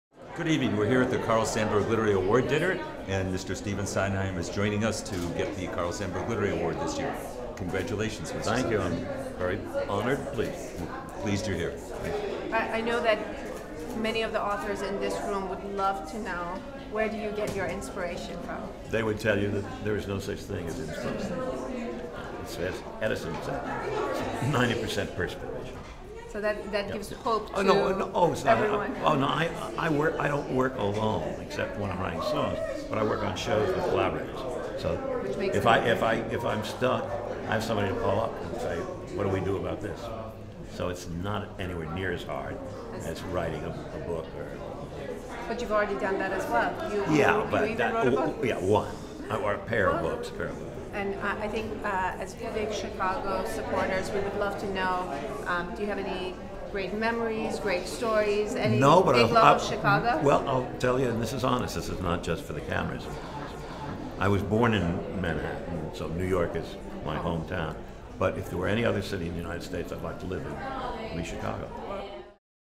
Bob Wislow & Dina Yaghmai Payvar with Stephen Sondheim
Sondheim receives the Carl Sandburg Literary Award on October 21. Presented by Dinner Co-Chairs Dina Yaghmai Payvar and Bob Wislow